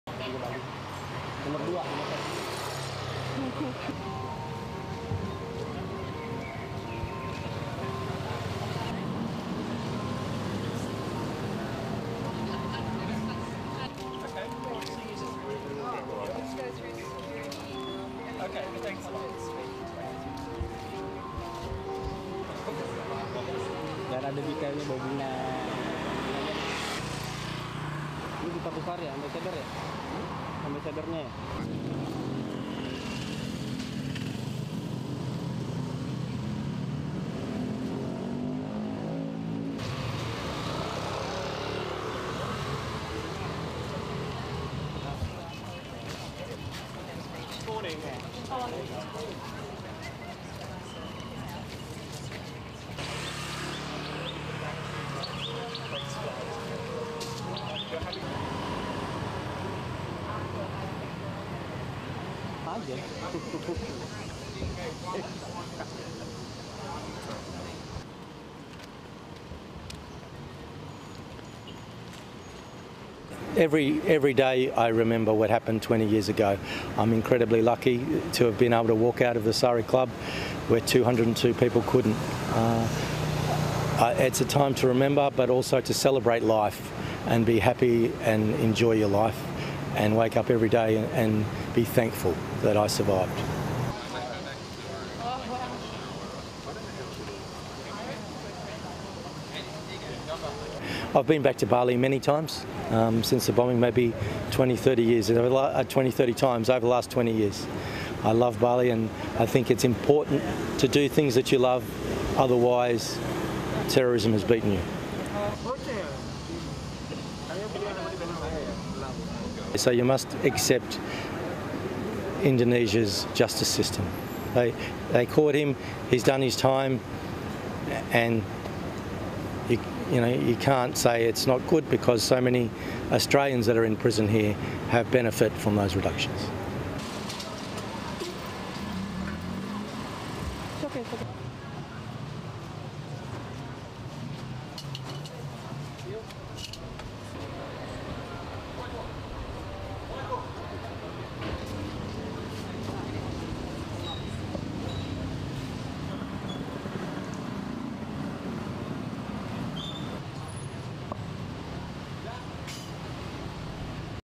Bali commemorates 20 years since bombings
RESTRICTION SUMMARY:
ASSOCIATED PRESS
Denpasar - 12 October 2022
1. Various of guests, officials and security outside Australian Consulate, exteriors of consulate
2. Various of guests leaving ceremony
3. SOUNDBITE (English) Glenn Cosman, survivor:
"Every day I remember what happened 20 years ago, I'm incredibly lucky to be able to walk out of Sari club where 202 people couldn't. It's a time to remember, but also time to celebrate life and be happy and enjoy your life and wake up everyday and be thankful that I survived."
4. Guests leaving
5. SOUNDBITE (English) Glenn Cosman, survivor:
"I've been back to Bali many times, since the bombing maybe 20 to 30 times over the last 20 years. I love Bali and I think it's important to do things that you love, otherwise terrorism is beating you."
6. Australian Officials
7. SOUNDBITE (English) Glenn Cosman, survivor:
"So you must accept Indonesia's justice system, they caught him, he's done his time and you can't say it's not good because so many Australians that are in prison here have benefitted by those reductions."
8. Various of people at Bali bombings memorial
STORYLINE:
Hundreds gathered on Wednesday on the Indonesian resort island of Bali to commemorate 20 years since a twin bombing killed 202 people, mostly foreign tourists, including 88 Australians and seven Americans.
Services were held simultaneously in several places in Australia and at Bali's Australian Consulate in the city of Denpasar, where Australian survivors of the 2002 terrorist attack and relatives of the deceased were among the 200 in attendance to pay tribute.
Survivors are still battling with their trauma from the Saturday night in October 2002, when a car bomb in Sari Club and a nearly simultaneous suicide bomb at nearby Paddy's Pub went off.
That night remains seared into the national memories of Indonesians, Australians and many others.
After the attack, the bustling tourist area was quiet for a time, but it has since returned to a state of busy weekends, packed traffic and tourists.
What used to be Sari Club is now a vacant lot, while Paddy's Pub has resumed its operation 100 meters (300 feet) from its original location.
A monument stands less than 50 metres (yards) from the bombing sites with the names of the those who died inscribed on it.
People regularly come to pray and place flowers, candles, or flags with photos of their loved ones.
In August, Indonesia's government considered granting an early prison release to the bombmaker in the Bali attack, Hisyam bin Alizein, 55, better known by his alias, Umar Patek, who has also been identified as a leading member of Jemaah Islamiyah.
Indonesian authorities said Patek was an example of successful efforts to reform convicted terrorists and that they planned to use him to influence others not to commit terrorist acts.
But the Australian government has expressed its strong opposition to his possible release.
One Australian survivor, who attended a service at Bali's Australian Consulate, said however that "you must accept Indonesia's justice system."
"They caught him, he's done his time and you can't say it's not good because so many Australians that are in prison here (in Bali) have benefitted by those reductions," Glenn Cosman said.
In 2020, 228 people were arrested on terrorism charges.
The number rose to 370 last year, underscoring authorities' commitment to pursue suspects even as the number of terrorist attacks in Indonesia has fallen.